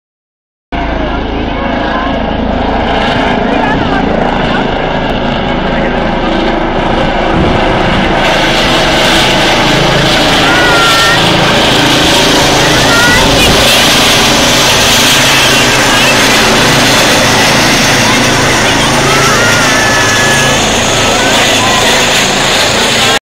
PSNI Helicopter ,Comber 2012
PSNI helicopter after take off .At Comber family day April 2012. Lots of grass being blown by downdraft. Missed take off ,took ages